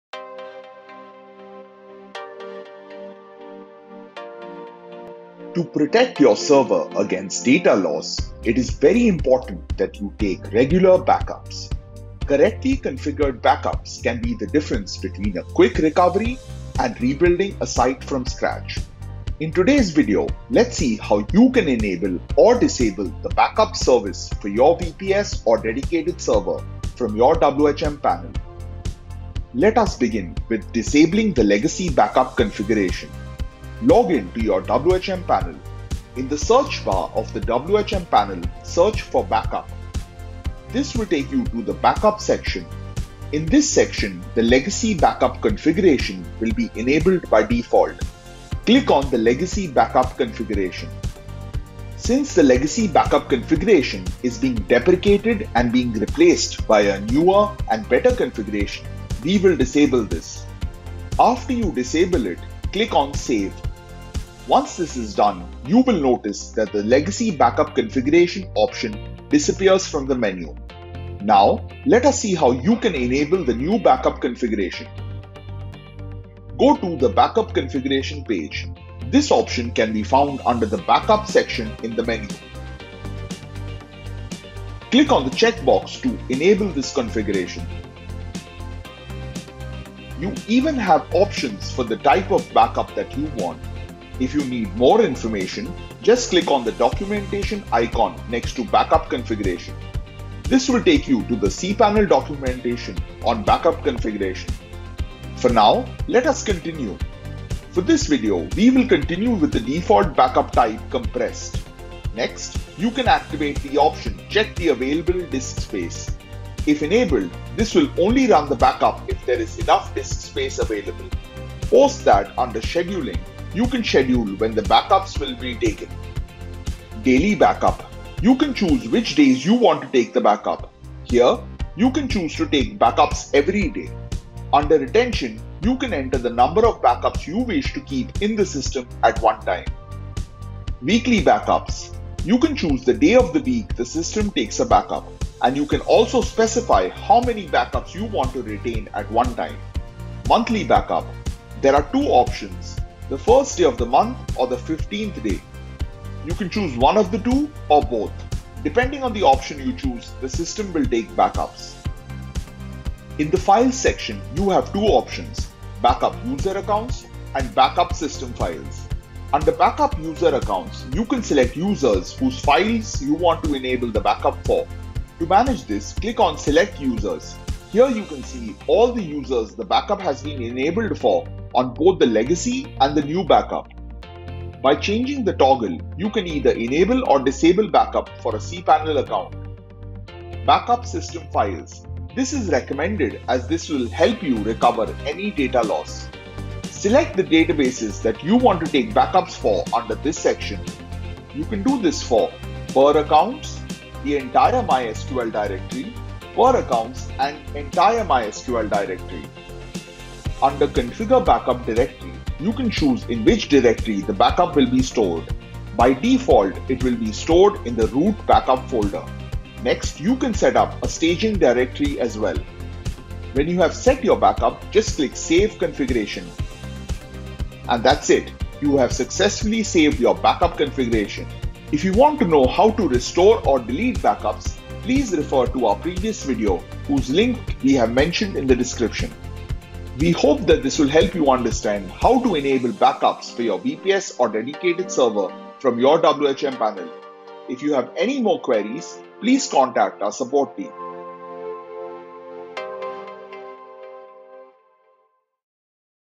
How to Enable and Disable Backup from WHM?
Building a website on WordPress is easy and if you are looking for WordPress hosting, why not look since 2005.

To protect your server against data loss, it is very important that you take regular backups. Correctly configured backups can be the difference between a quick recovery and rebuilding a site from scratch.

In today’s video, lets see how you can enable or disable the backup service for your VPS or Dedicated server from your WHM panel.

Let us begin with Disabling the Legacy Backup Configuration
- Log in to the WHM panel. In the search bar of the WHM panel, search for “Backup”. This will take you to the backup section. In this section, the “Legacy Backup Configuration” will be enabled by default.

- Click on the “Legacy Backup Configuration”. Since the legacy backup configuration is being deprecated and being replaced by a newer & better configuration - We will disable this.
After you disable it, click on Save. Once this is done, you will notice that the Legacy Backup Configuration option disappears from the menu.

- Now let us see how we can enable the New Backup Configuration. Go to the “Backup Configuration” page. This option can be found under the “Backup section” in the menu.

- Click on the check-box to enable this configuration. You even have options for the type of backup that you want. If you need more information, just click on the documentation icon, next to Backup Configuration. This will take you to the cPanel documentation on Backup Configuration.

- For now, let us continue. For this video, we will continue with the default Backup type  “Compressed”

- Next, you can activate the option “Check the Available Disk Space”. If enabled, this will only run the backup if there is enough disk space available.

 - Post that, under scheduling, you can schedule when the backups will be taken.

Daily Backup - You can choose which days you want to take the backup. Here, you can choose to take backups every day. Under retention, you can enter the number of backups you wish to keep in the system at one time.

Weekly Backups - You can choose the day of the week the system takes a backup. And you also can specify how many backups you want to retain at one time.

Monthly backup - There are two options, the first day of every month or the 15th day. You can choose one of the two, or both. Depending on the option you choose, the system will take backups.

- In the Files section you have two options
1.Backup User Accounts
2.Backup System Files

1.Under, “Backup User Accounts” you can select users whose files you want to enable the backup for. To manage this, click on “Select Users”. Here you can see all the users the backup has been enabled for on both the legacy and the new Backup. By changing the toggle you can either enable or disable backup for a cPanel account.

2.Backup System files- this is recommended as this will help you recover any data loss. Select the databases that you want to take backups for under this section. You can do this for:
1.Per Accounts
2.The entire MySQL Directory
3.Per Accounts and entire MySQL Directory

- Under “Configure Backup Directory, you can choose in which directory the backup will be stored. By default, it will be stored in the root backup folder.

- Next, you can set up a staging directory as well.

When you have set your backup just click Save Configuration, and that's it! You have successfully saved your backup configuration.

If you want to know how to restore or delete backups, please refer to our previous video, whose link we have mentioned in the description.

We hope that this will help you understand how to enable backups for your VPS or Dedicated server from your WHM panel.